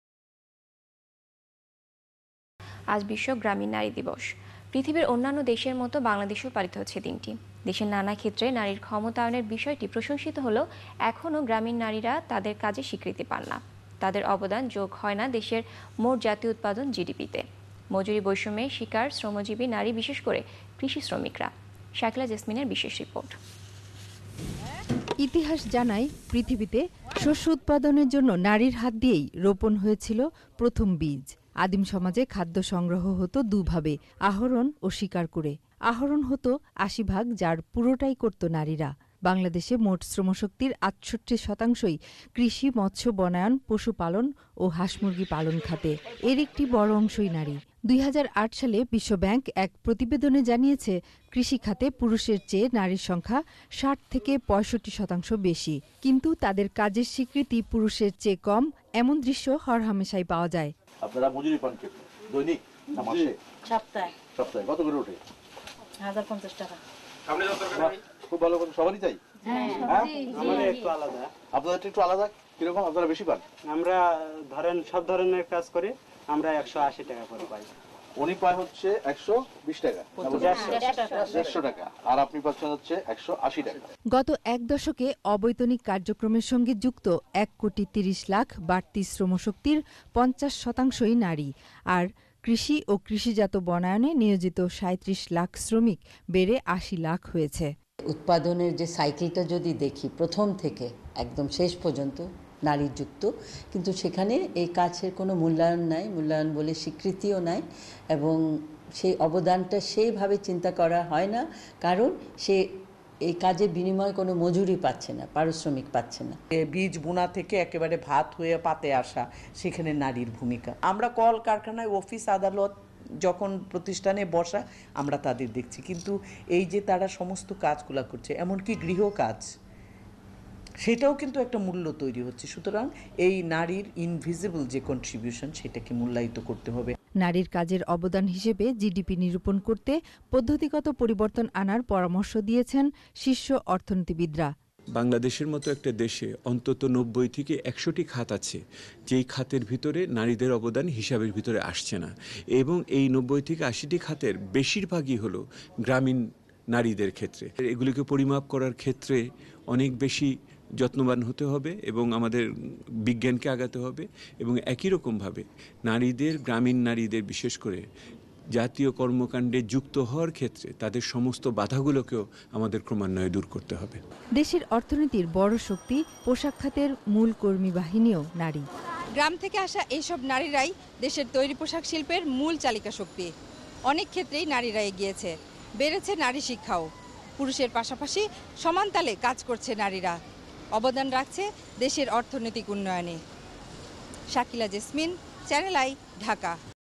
বিশ্ব গ্রামীন নারী দিবস আজ
বিশ্ব গ্রামীন নারী দিবস আজ, নারীর ক্ষমতায়নের বিষয়টি প্রশংসিত হলেও গ্রামীণ নারীরা তাদের কাজের স্বীকৃতি পান না, মজুরি বৈষম্যের শিকার নারী কৃষি শ্রমিকরা।